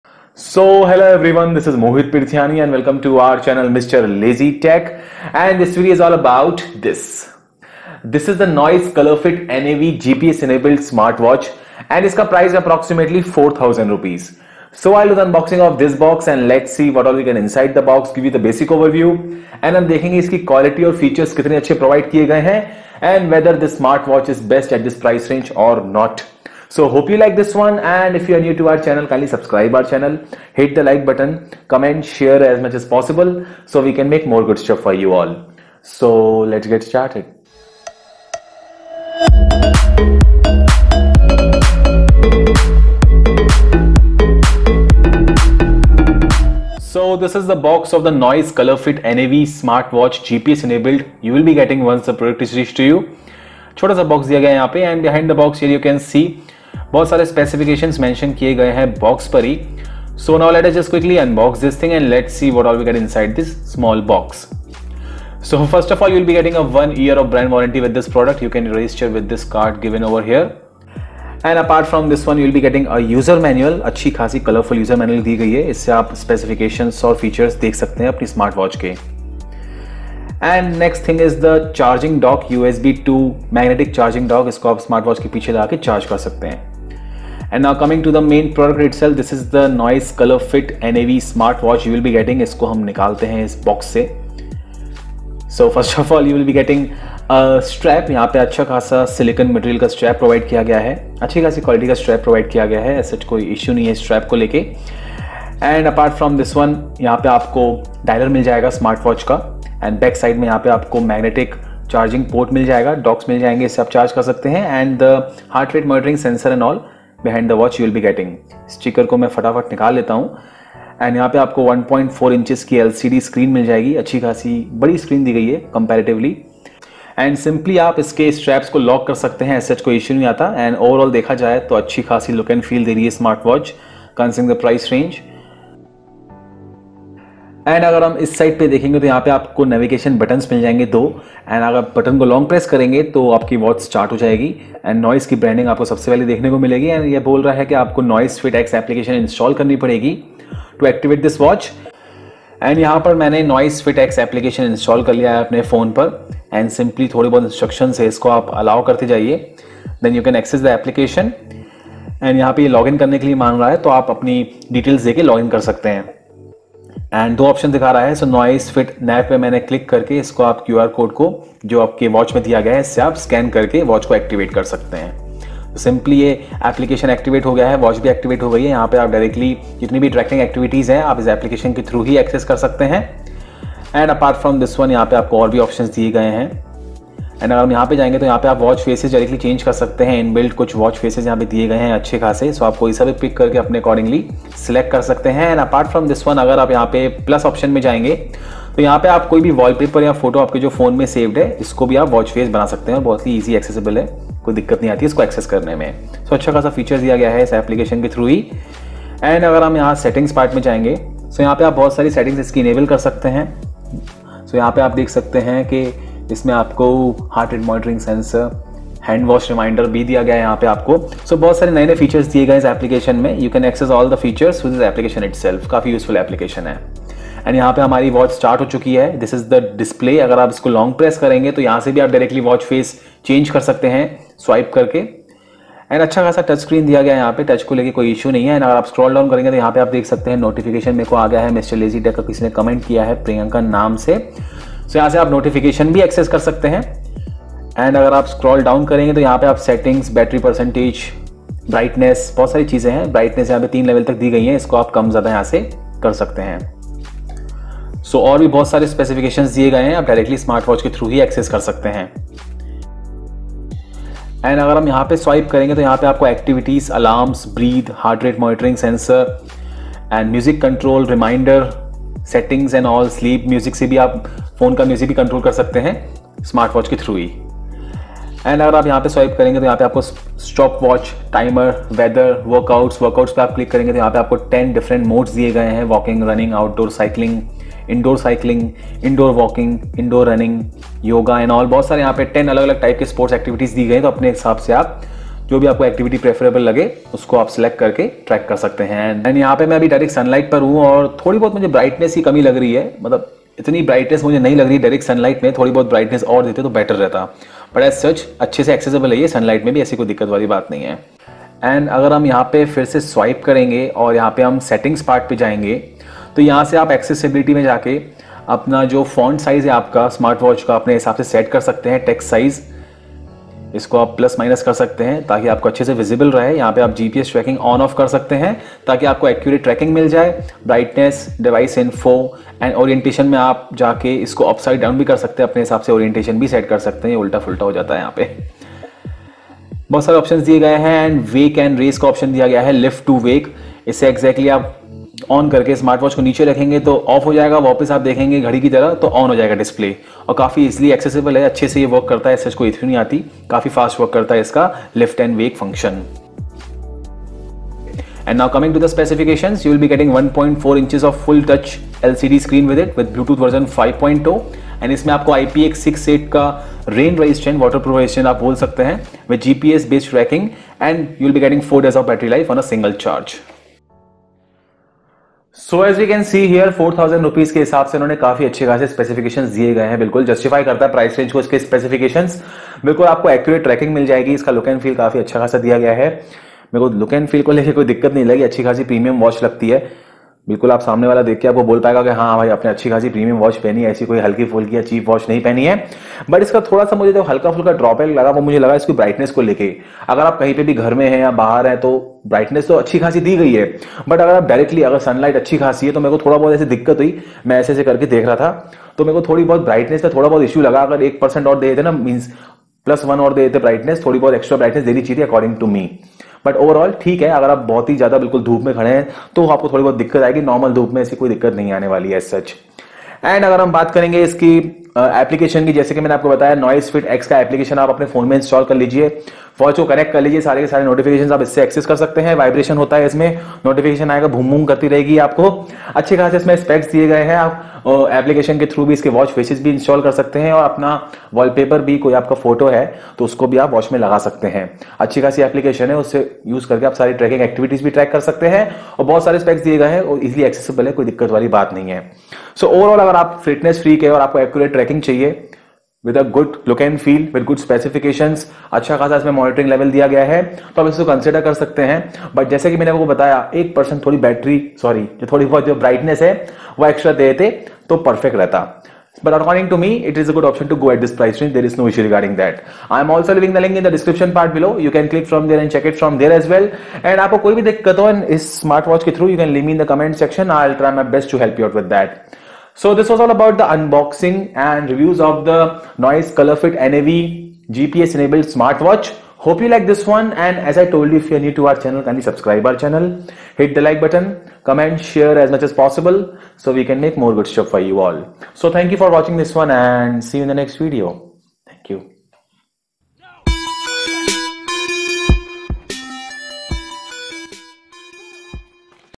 Noise ColorFit NAV Smart Watch with Built-in GPS ⌚ Best Smart Watch Under 4000 🤷‍♂️🤷‍♂️
Noise ColorFit NAV Smart Watch with Built-in GPS UNBOXING / Reviews ⌚ Best Fitness SmartWatch Under 4000 🤷‍♂️🤷‍♂️

This Video is all about "Noise ColorFit NAV Smart Watch with Built-in GPS UNBOXING / Reviews ⌚ Best Fitness Smart Watch Under 5000 🤷‍♂️🤷‍♂️"

Noise ColorFit NAV Smart Watch with Built-in GPS :-

Laptop Mouse :-

Mobile Phone :-